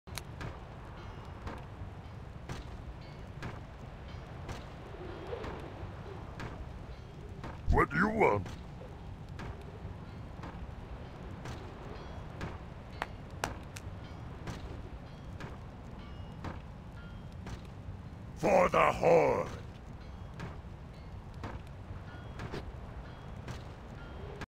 Lift with the legs
My lumberjack does some knee bends to stay fit.